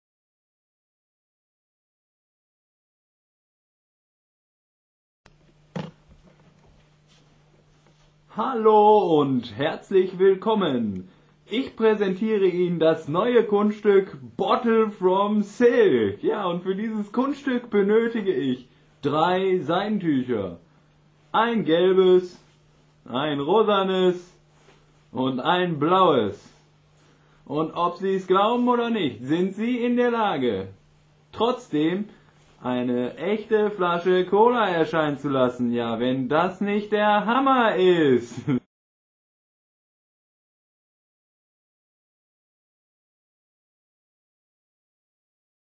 Produzioni dai foulards di bottiglia o fiori
Un grande gioco di foulard per gli amanti della magia da scena.

Il prestigiatore tiene in mano foulards di colori differente. Uno alla volta li lancia in aria per dimostrare che non nascondono nulla. Quindi, istantaneamente produce dagli stessi una bottiglia di spumante e a seguire un bouquet di fiori. Il pubblico non crederà ai propri occhi! Nelle feste allo spumante ci pensa il mago!

• È perfetto per qualsiasi esibizione, e semplicissimo da fare.

I foulard misura 70x30cm sono di colore:

VERVE

GIALLO

BLU

ROSSO (due pezzi di cui uno è il gimmick)

Bianco 50x50

 Oltre alla bottiglia potrete produrre una colomba, un piccolo oggetto, o un bouquet di fiori come nel video.